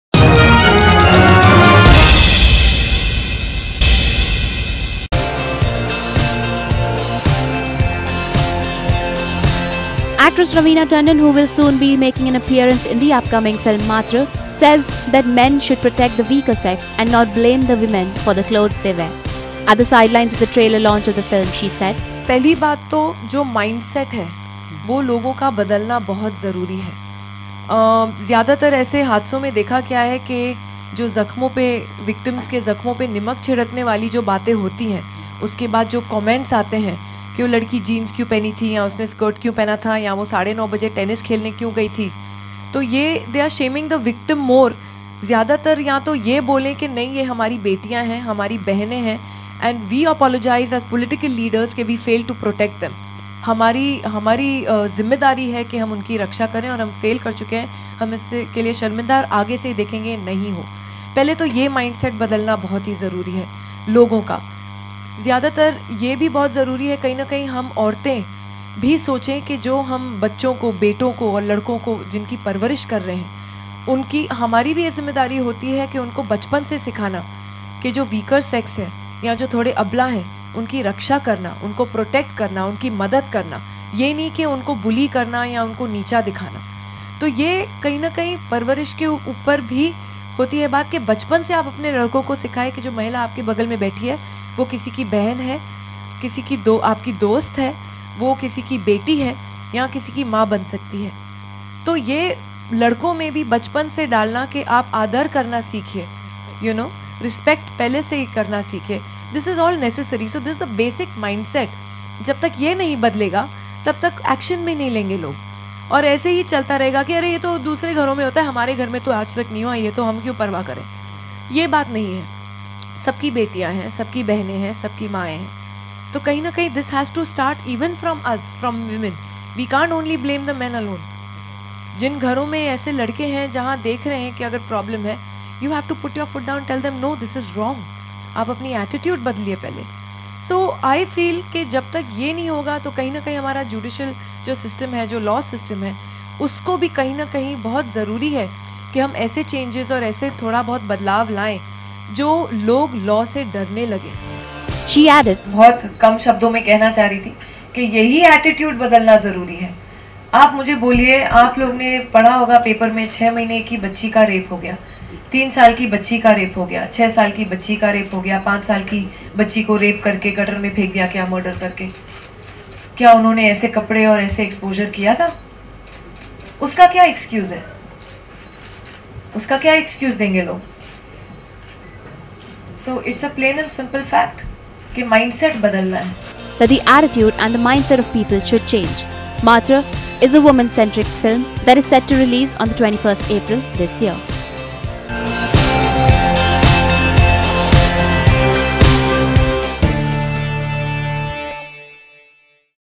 Protect the 'Abla Nari': Raveena Tandon
Actress Raveena Tandon who will soon be making an appearance in upcoming film 'Maatr' says that men should protect the weaker sex and not blame the women for the clothes they wear.